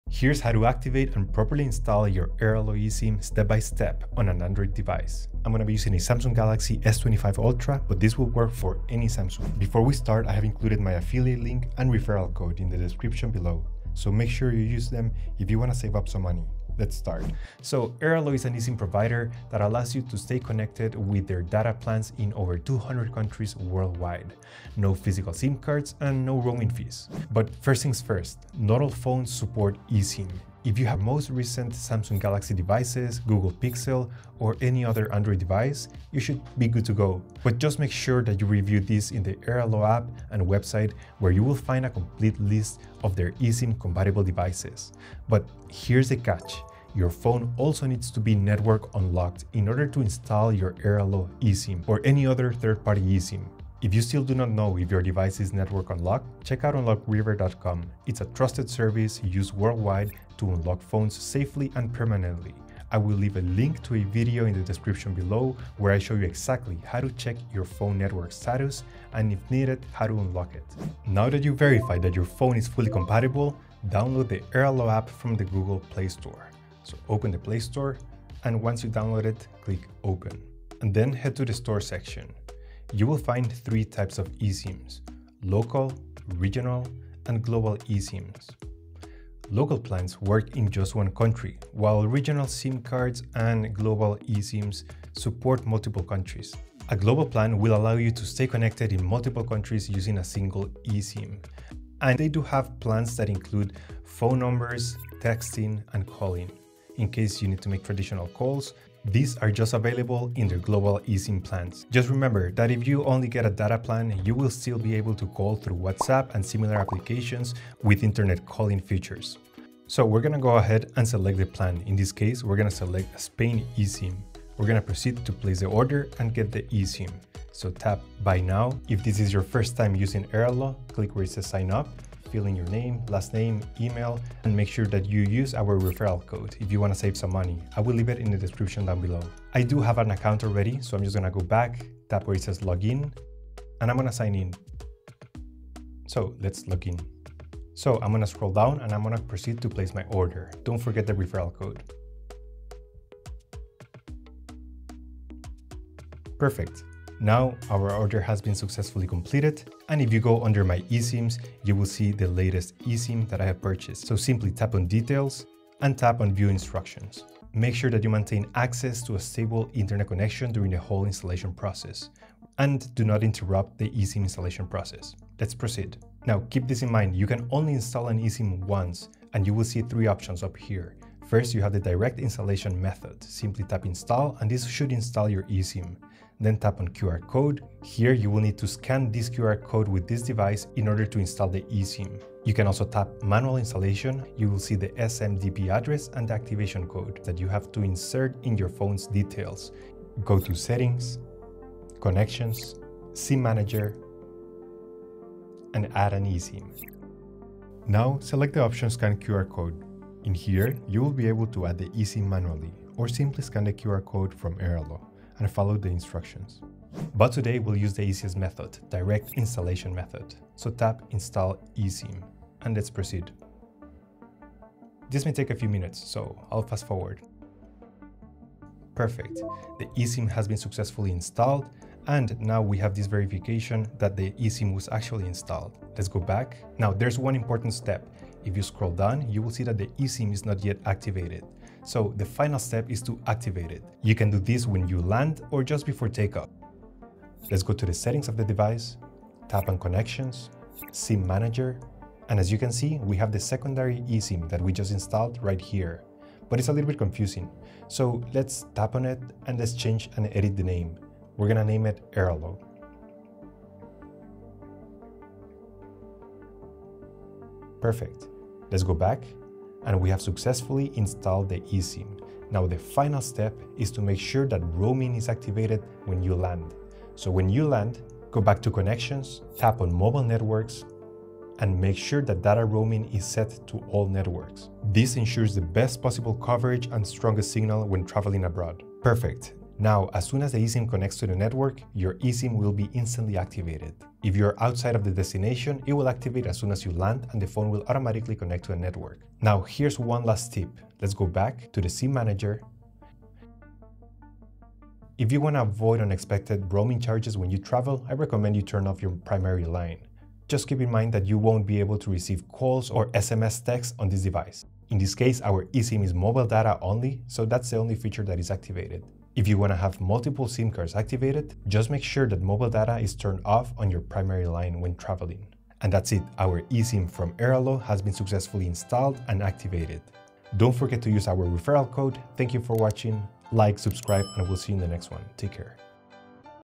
Airalo eSIM Activation Android | Step-by-Step Setup for Samsung, Pixel & All Devices
Activate your Airalo eSIM on any Android phone with this easy step-by-step guide!
Whether you're using a Samsung Galaxy, Google Pixel, Huawei, or any other Android device, this tutorial covers everything from eSIM compatibility checks to full installation and activation on Airalo.
Don't forget to make sure your Android is eSIM compatible and network unlocked before purchasing any eSIM.

📲  In this guide, you'll learn:
• How to check if your phone supports eSIM
• How to buy, install & activate your Airalo eSIM (QR code, Manual, or Direct Method)
• Tips to avoid issues with mobile data, roaming & APN setup

📱 Compatible with Android phones like:
• Samsung Galaxy S23, S22, S21, S24, S25, Fold Series, Flip Series, A series and much more.
• Google Pixel 3 to Pixel 8
• Huawei P40 Pro and more

⏰ Timestamps
00:00 Intro: Airalo eSIM Activation Android
00:09 Affiliate link & Referral code
00:27 Is Your Android Phone eSIM Compatible?
01:10 Airalo App download & plan overview from
02:00 Buying an eSIM from Airalo
02:47 How to install your Airalo eSIM on Android (QR, Manual, Direct Options)
03:55 Activating eSIM & Enabling Data Roaming
04:59 Final Setup, Tips & Troubleshooting
05:30 Outro – Airalo eSIM Activation Android